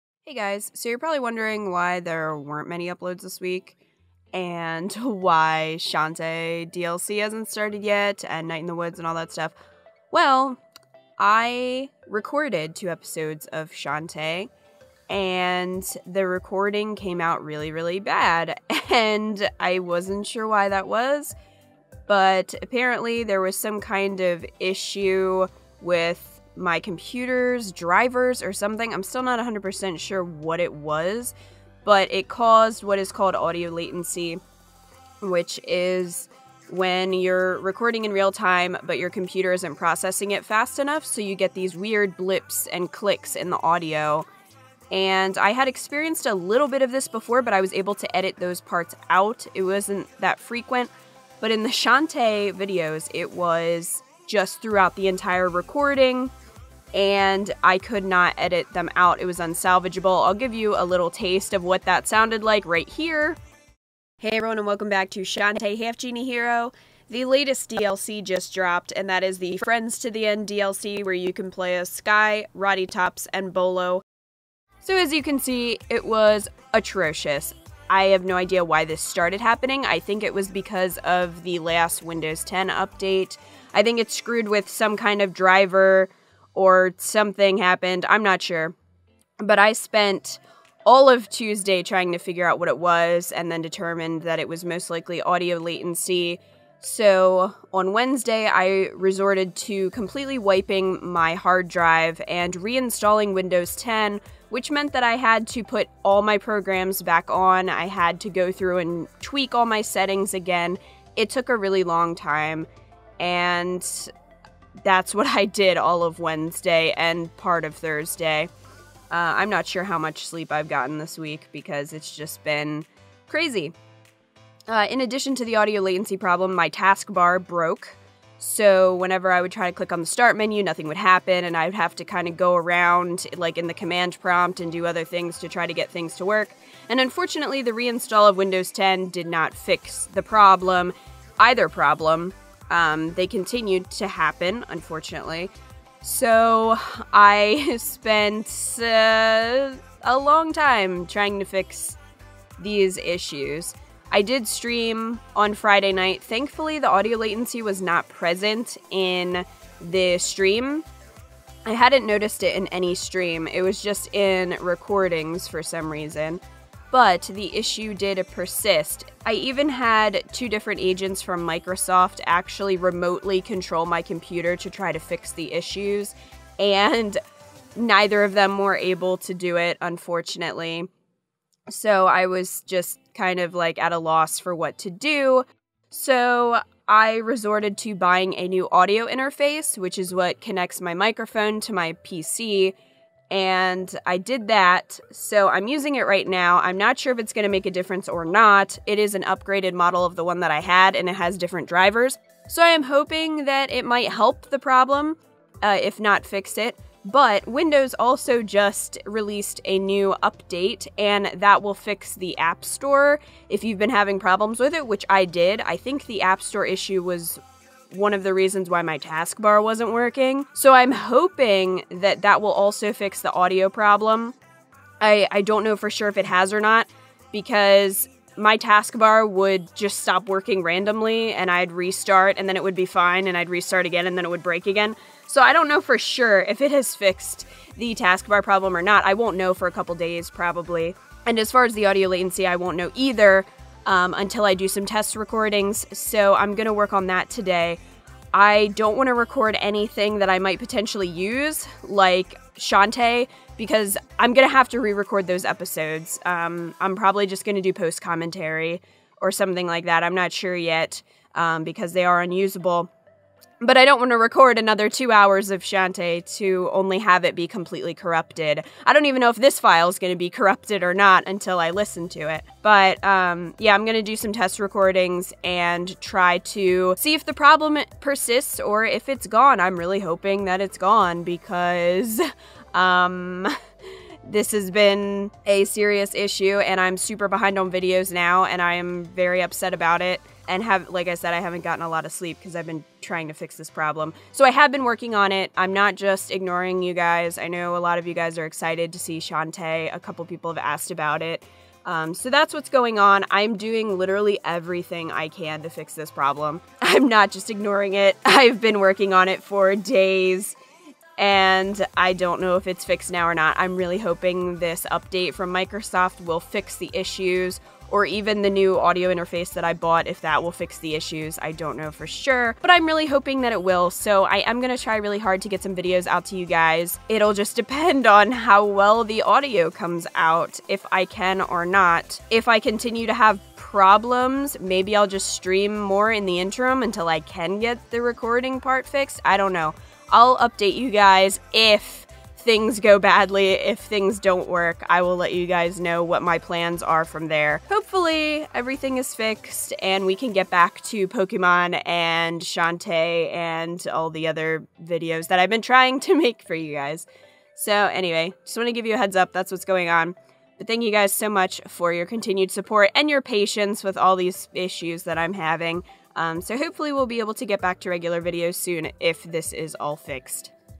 My Computer Hates Me ~ Tech Issues, Shantae DLC, Lack of Videos
As always, thanks for your patience! ♥

☼KITA NASH ON SOCIAL MEDIA☼

Artwork done by Echoii